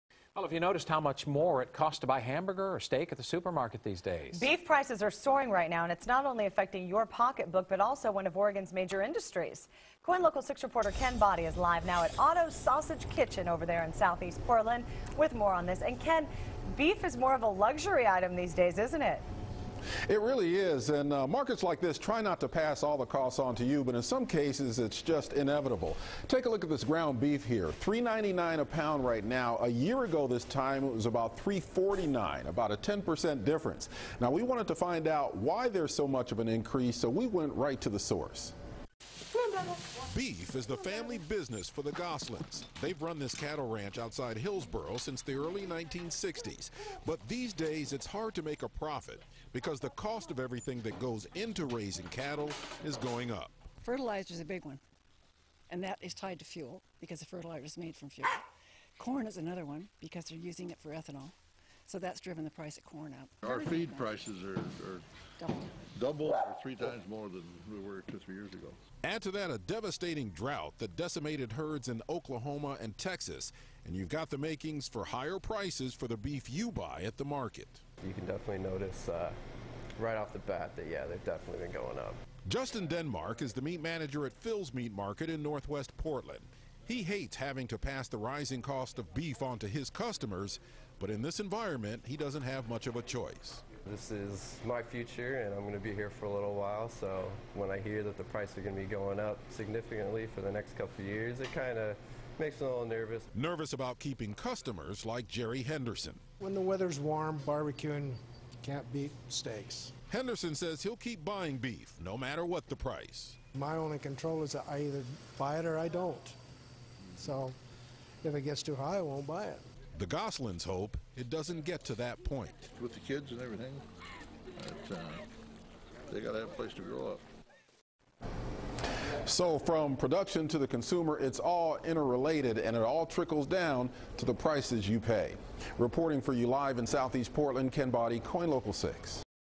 Rising beef prices could impact Oregon's economy
It is likely costing you more to buy that hamburger, beef roast or steak at the supermarket these days.